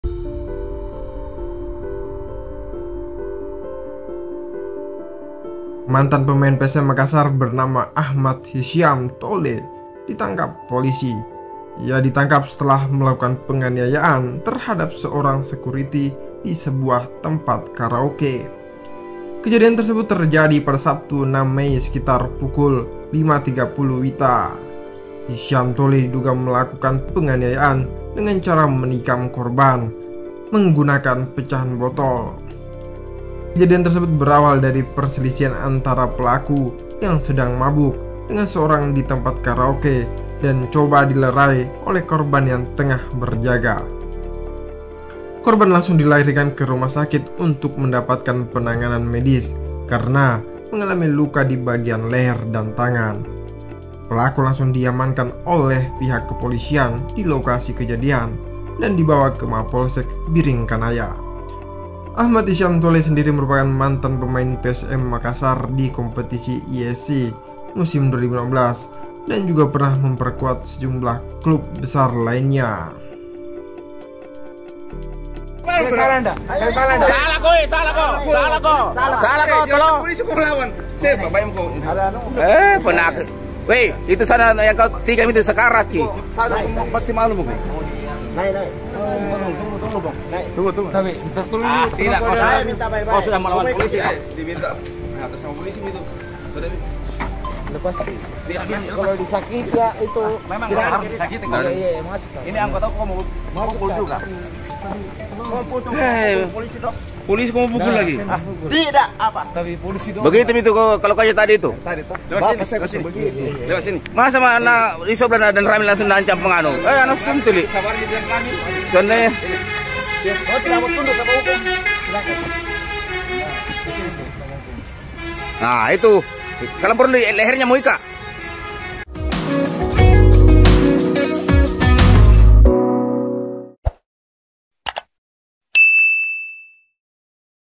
Mantan Pemain PSM Makassar Ditangkap Usai T!k4m Sekuriti
Mantan pemain PSM Makassar bernama AchmadHisyam Tolle ditangkap polisi.

Ia ditangkap setelah melakukan peng4n!ayaan terhadap seorang sekuriti di sebuah tempat karaoke.

Kejadian tersebut terjadi pada Sabtu (6/5) sekitar pukul 05.30 WITA.

Achmad Hisyam Tolle sendiri merupakan mantan pemain PSM Makassar di kompetisi ISC musim 2016 dan juga pernah memperkuat sejumlah klub besar lainnya seperti Borneo FC, Mitra Kukar, PSS Sleman, hingga PSIM Yogyakarta.

Video terkini dan terbaru..